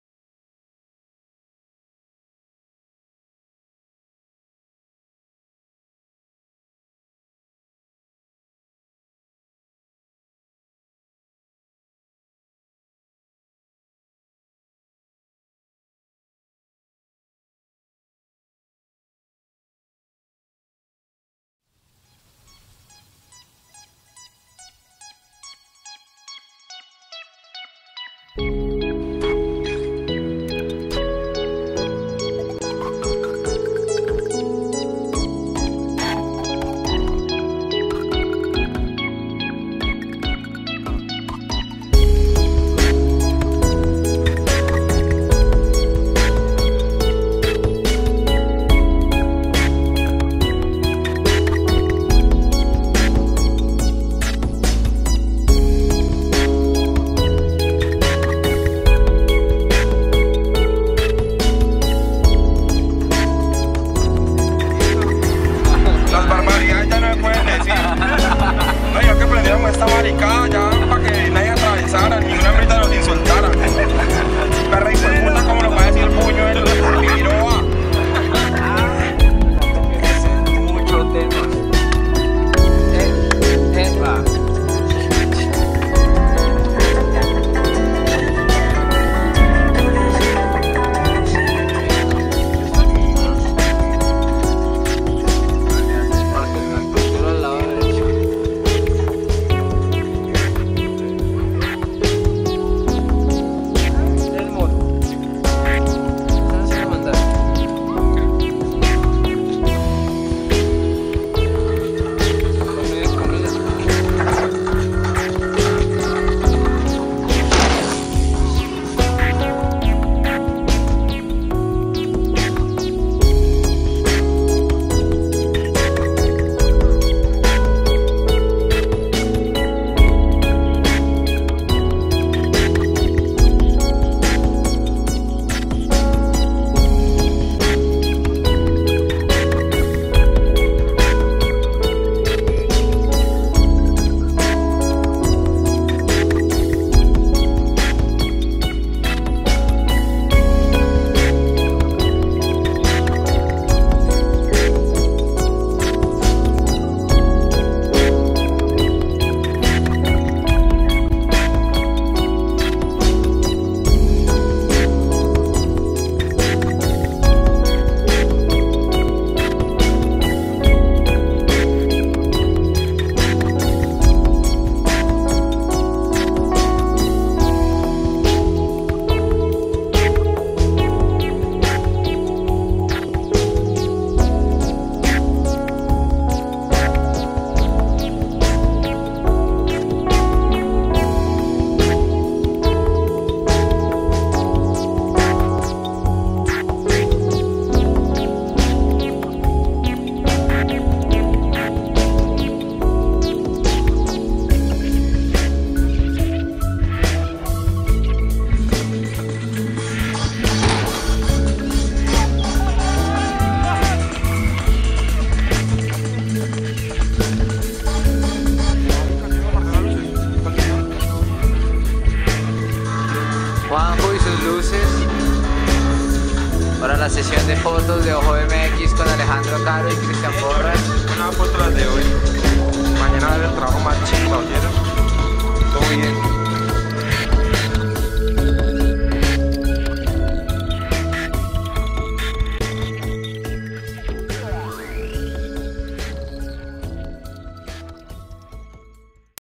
OJOBMX - De visita en la finca de Alejandro Caro Cap 1
En la 2da semana de enero del 2011, estuvimos de visita en la finca de Alejandro Caro, para filmar un fin de semana de entrenamiento con el y Cristian Porras.
El material que se recogió fue bastante, así que decidimos dividirlo en dos ediciones diferentes. Esperamos que disfruten la primera de ellas.
Agradecemos a Alejandro Caro por la invitación y hospitalidad que nos brindo en nuestro viaje al Valle en enero 2011